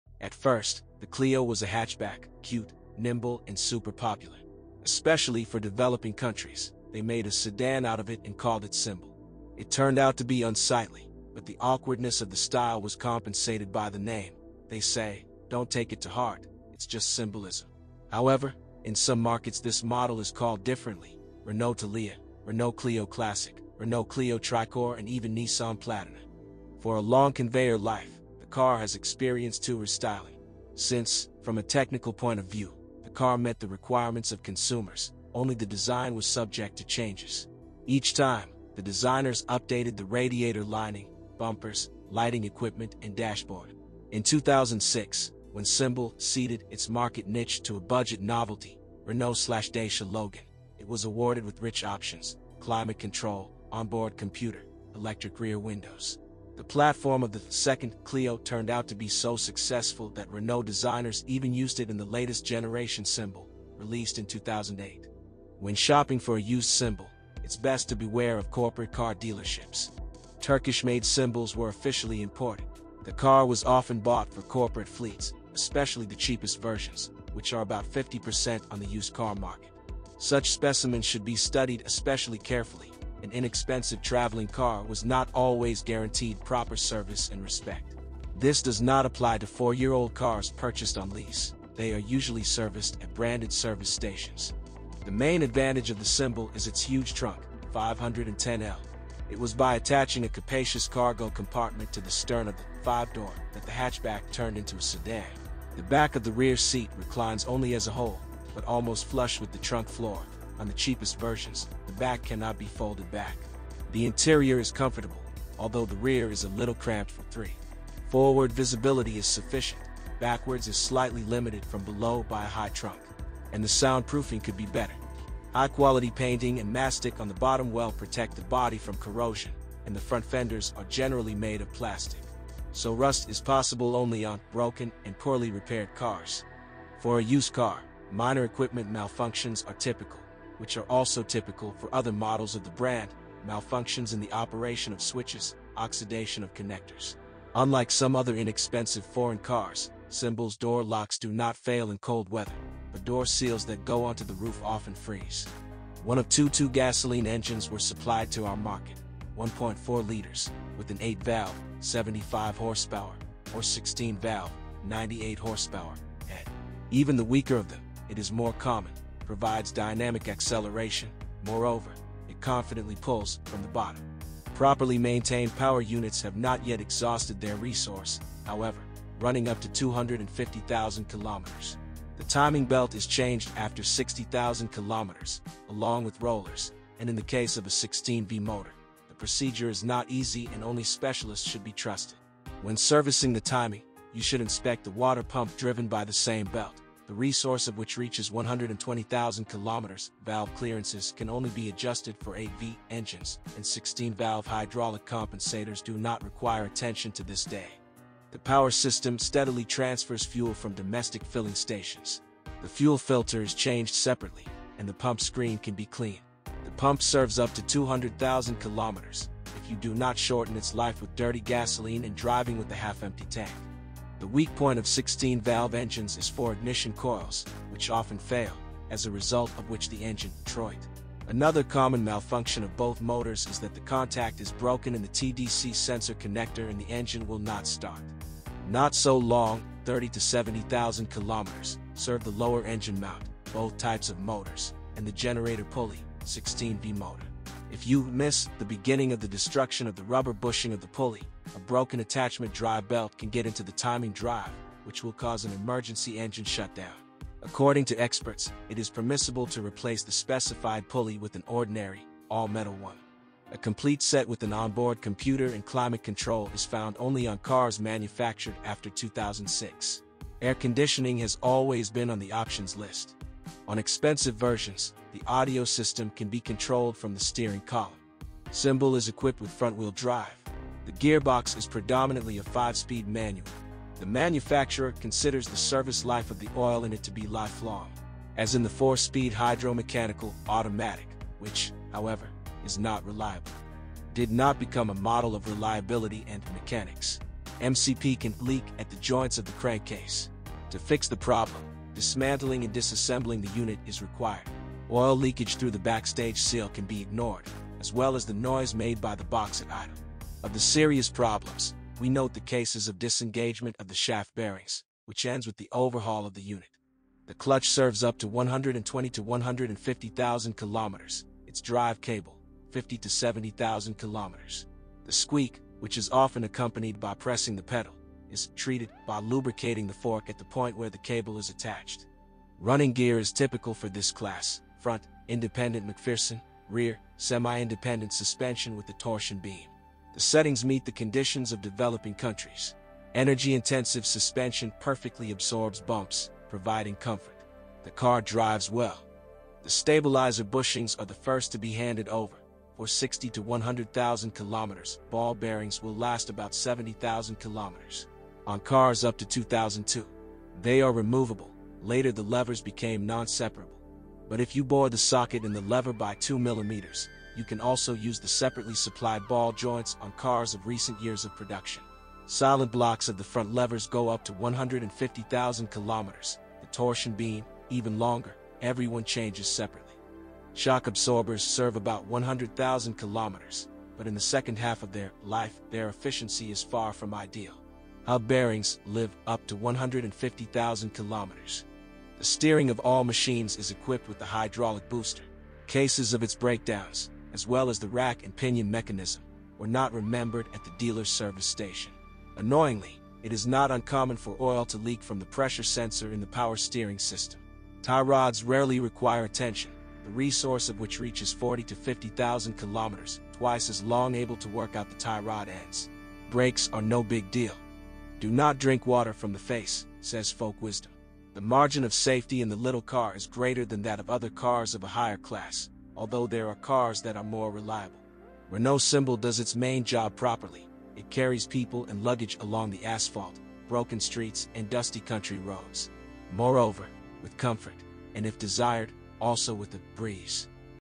Everything You Need to Know About the Renault Symbol 1 generation - Fault Guide
The most common problems with a used Renault Symbol I generation?